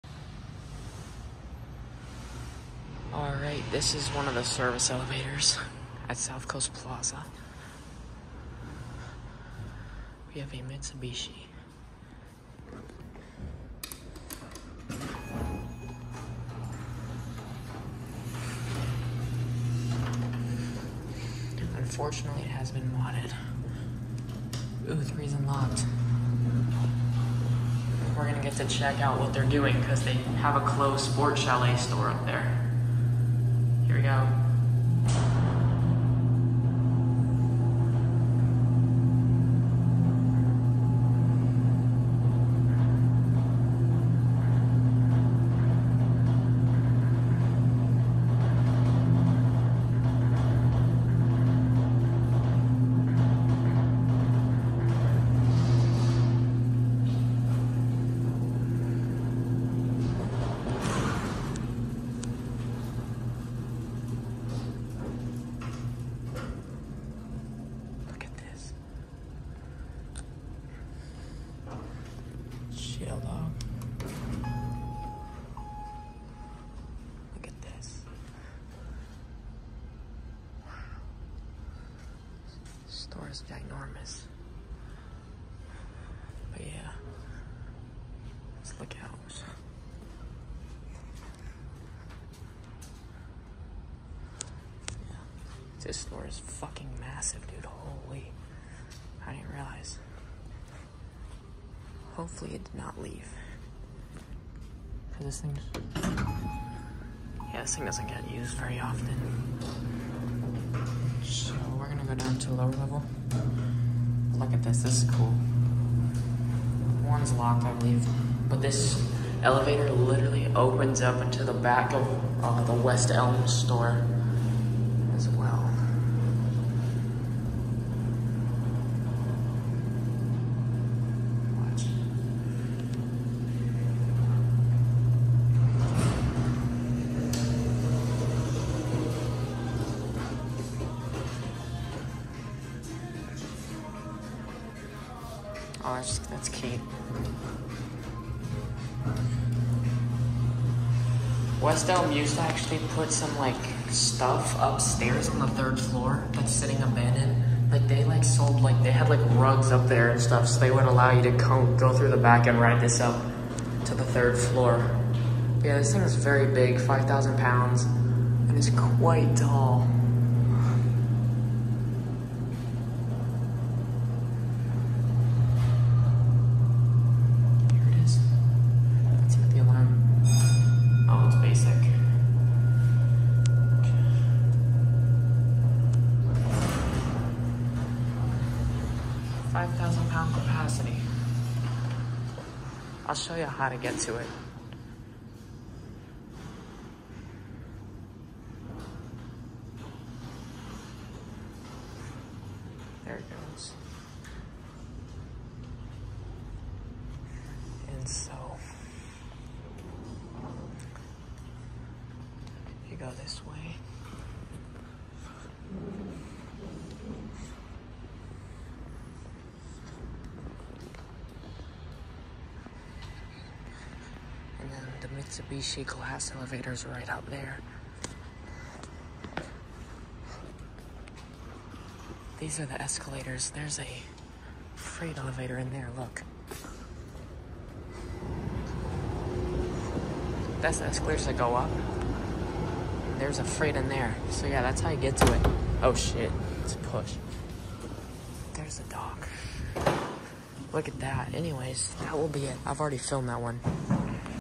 A Mitsubishi Service Elevator that goes to an Abandoned Store at South Coast Plaza in Costa Mesa, CA
This service elevator is on the small side of South Coast Plaza and serves the Lower Level, Anthropologie, West Elm, and the old Sport Chalet that is now closed. However, it was unlocked so I was able to see the abandoned Sport Chalet store that has been sitting like this for years. This elevator is frequently used by West Elm as well. But yeah this is a Mitsubishi modernized with ERM, so the elevator itself isn’t interesting.